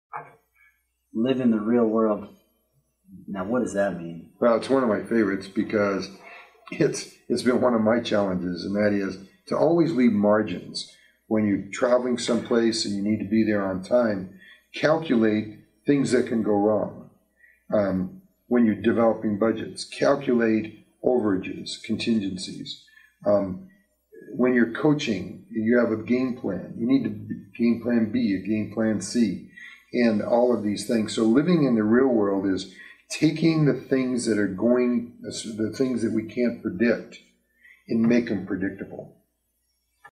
LOOPS - Live in the Real World
LOOPS4Biz.com
Interview with Mike Chaet co-author of the book  LOOPS  the Seven Keys to Small Business Success, answering the question "What does it mean to lve in the real world?"